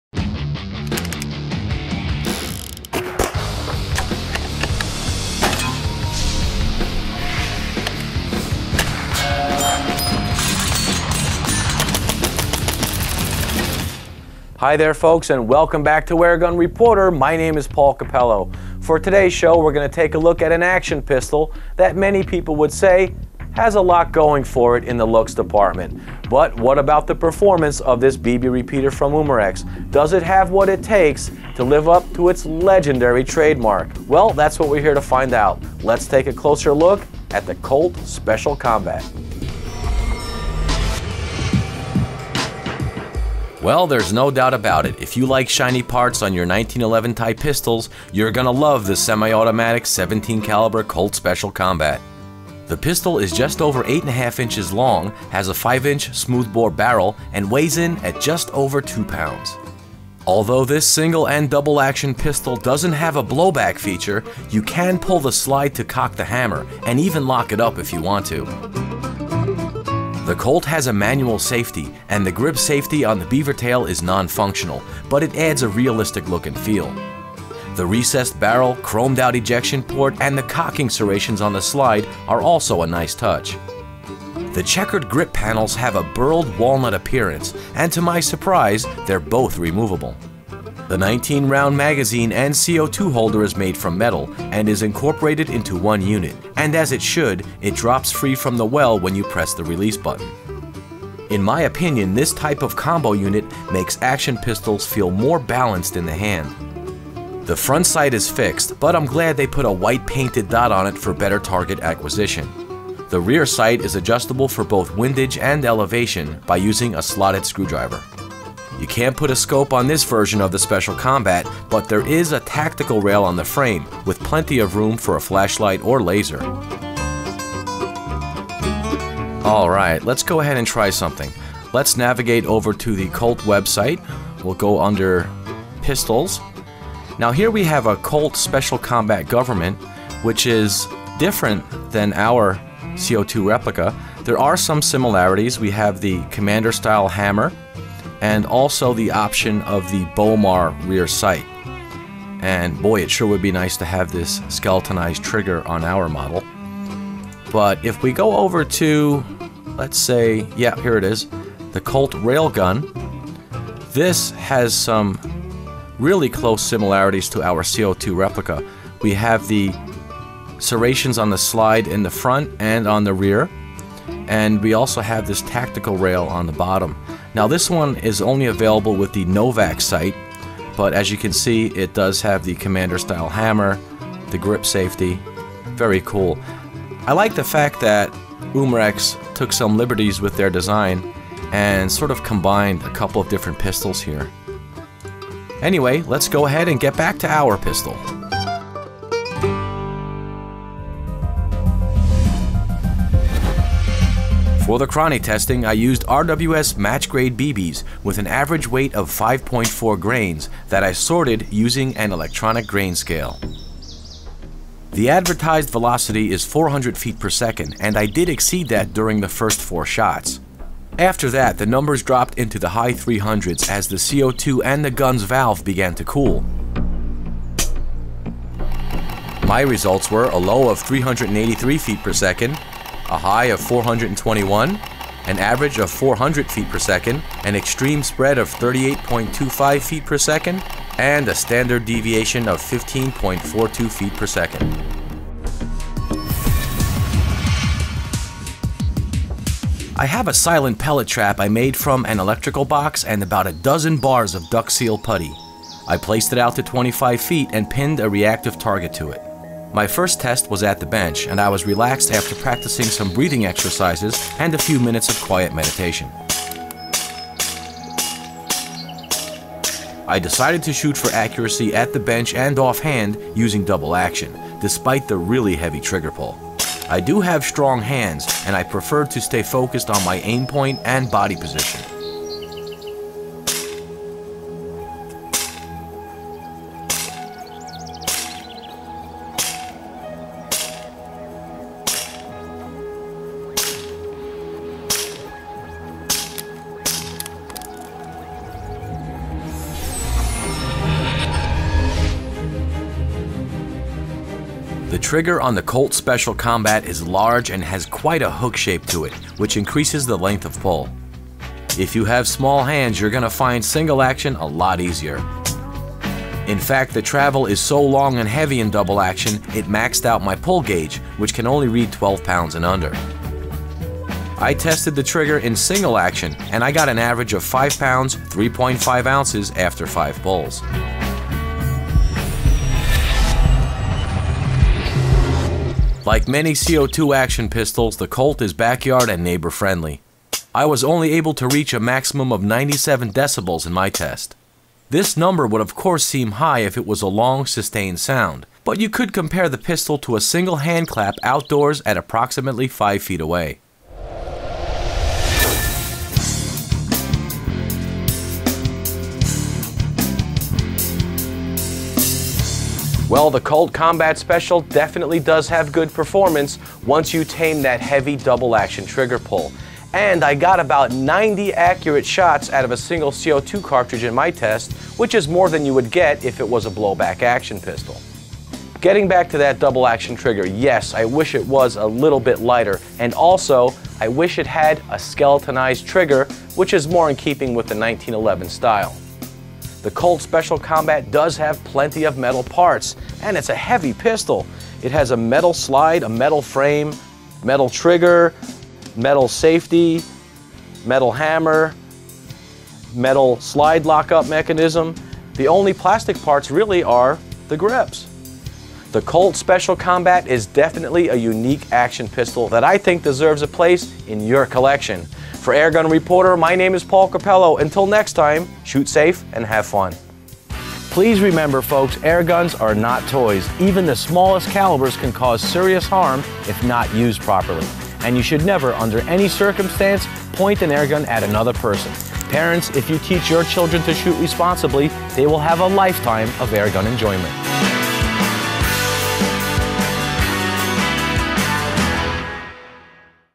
Colt Combat Special CO2 gun - AGR Episode #84
Paul takes a look at this new CO2 gun...